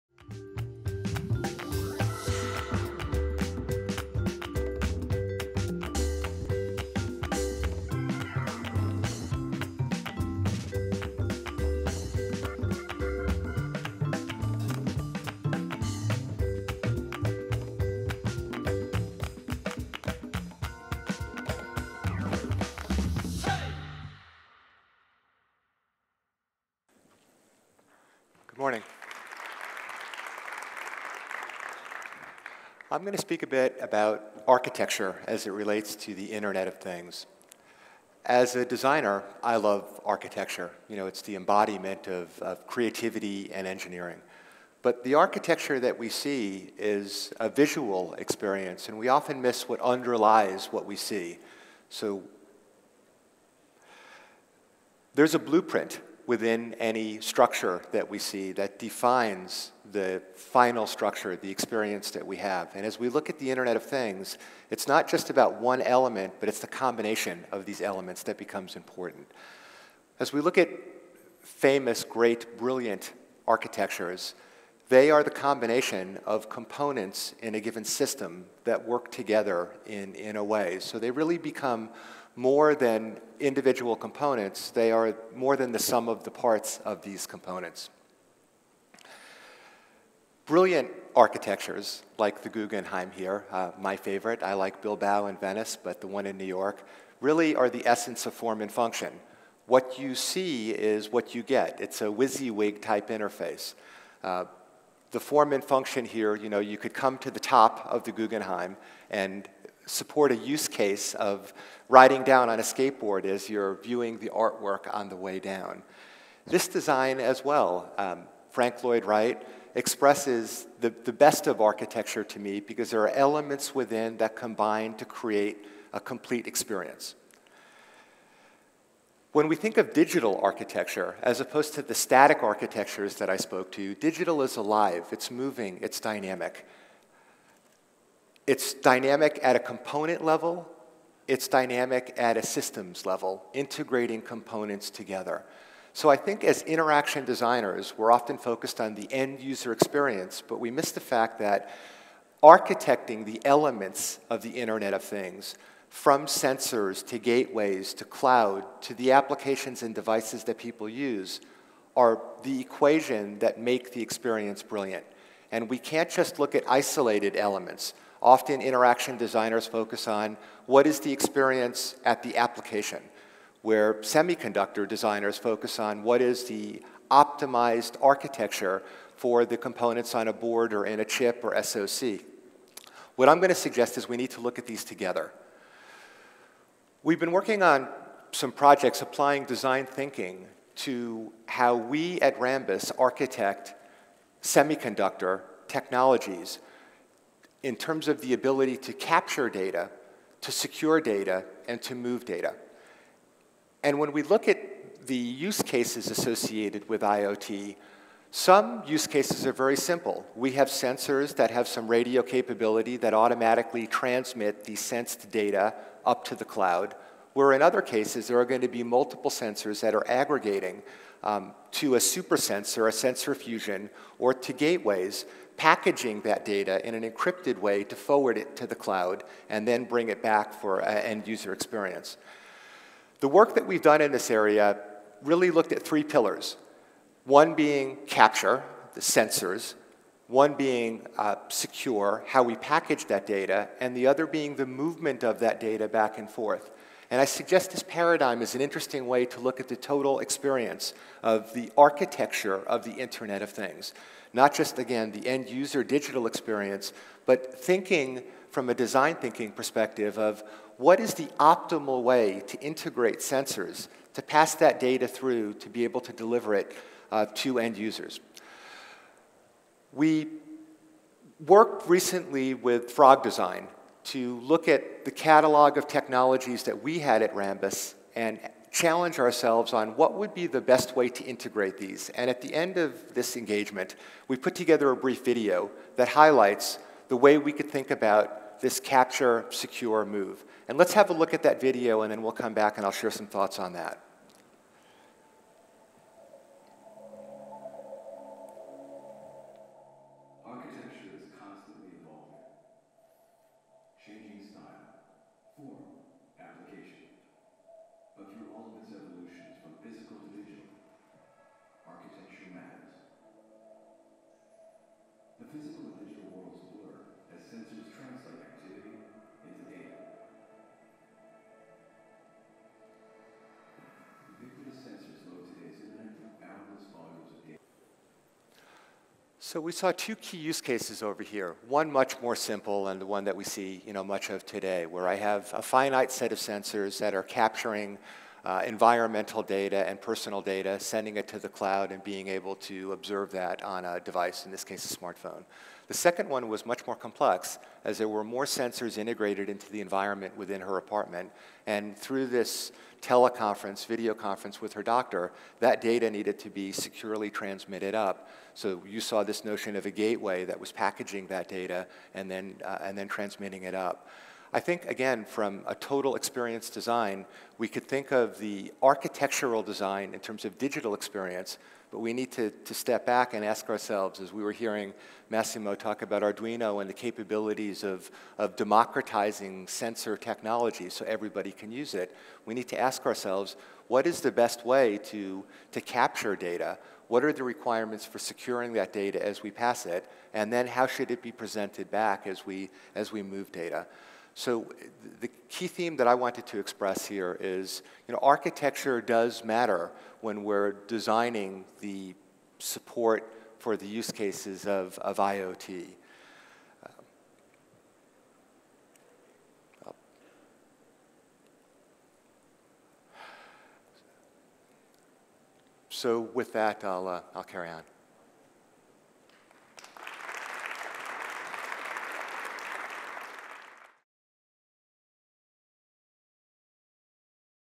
Internet of Things. Jèrôme Nadel (Rambus) - 4 Years From Now
Keynote previous to the Panel "Internet of Things" with Jèrôme Nadel, Senior Vice President & Chief Marketing Officer in Rambus. This keynote took place at The Audi Stage. 4YFN is an event of Mobile World Capital Barcelona.